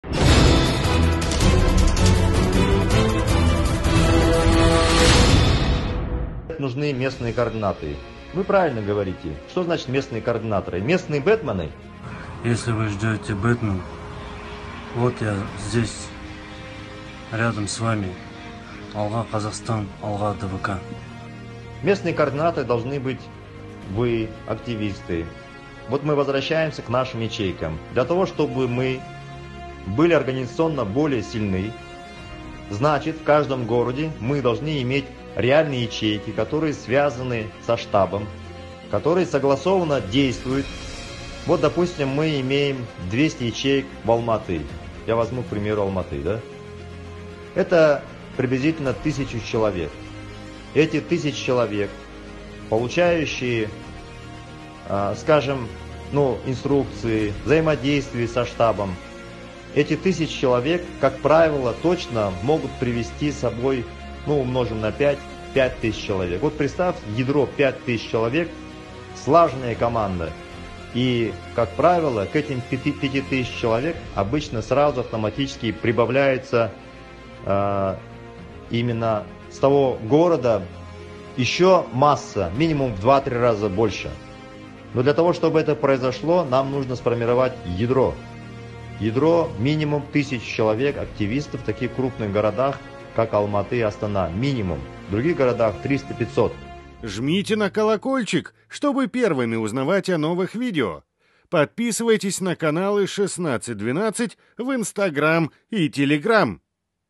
СОЗДАНИЕ ЯЧЕЙКИ ПОДГОТОВКА К ПОБЕДЕ РЕЖИМА НАЗАРБАЕВА
Уважаемые друзья!
Да, мы всю нашу жизнь находились под гнётом авторитаризма Назарбаева, подавляющего волю народа и личности, искажающего видение действительности и внушающего нам, что мы беспомощны и несамостоятельны.

Но всё же каждый из нас желает жить достойно, свободно и благополучно. А значит — желает перемен. Ознакомившись с программами ДВК, каждый получит достаточно ясное представление о целях движения и сможет аргументировать необходимость перемен (программы ДВК

Для смены режима Насанжора Назарбаева нужны массовые митинги! Организация массовых митингов требует координации действий большого количества людей. С этой целью была разработана стратегия создания ячеек. Ячейка — группа единомышленников численностью от 5 человек и выше. У каждого члена ячейки есть свой круг общения.

Если вы уже осознали необходимость смены власти и вооружены знаниями о наших программах, создайте и вы свою ячейку. Делать это надо уже сейчас — время не ждёт.

Если вы ещё не состоите в ячейке, ознакомьтесь с правилами и принципами создания ячеек на нашем телеграм-канале ВСЁ О ЯЧЕЙКАХ

После того, как вы соберёте ячейку, свяжитесь со штабом ДВК. Мы организуем для вашей ячейки онлайн-конференцию. Мы будем обсуждать план дальнейших совместных действий по смене власти.

Ответственность за наше бездействие уже ложится на каждого из нас. Объединившись мы вернём контроль в свои руки.

АЛГА, КАЗАХСТАН!